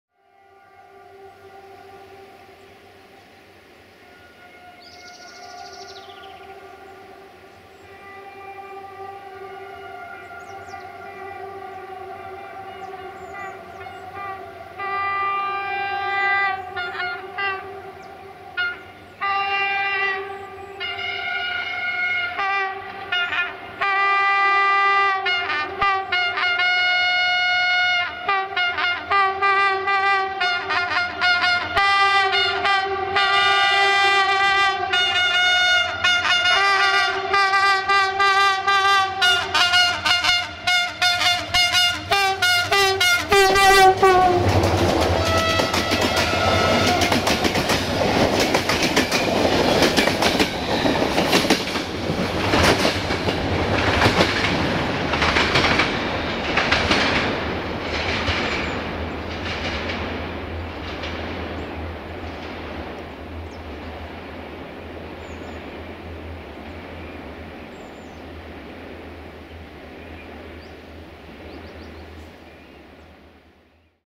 Mega Horns Show by Isabella - Jimmy 65-0950-9 in Apuseni Mountains - 28 February 2014
1:IR363-2"Biharia" - Budapest Keleti - Cluj Napoca - Jimmy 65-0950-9 - Isabella

28 February 2014